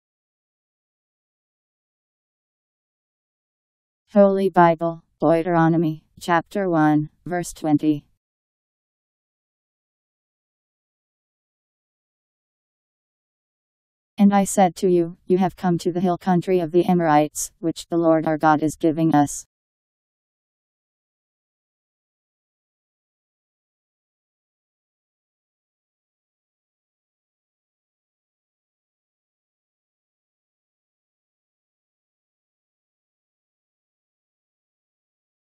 Holy Bible Deuteronomy Chapter 1 Verse 20
Video shows Holy Bible Deuteronomy Chapter 1 Verse 20: And I said to you, You have come to the hill-country of the Amorites, which the Lord our God is giving us. Powered by MaryTTS. Text taken from Bible in Basic Edition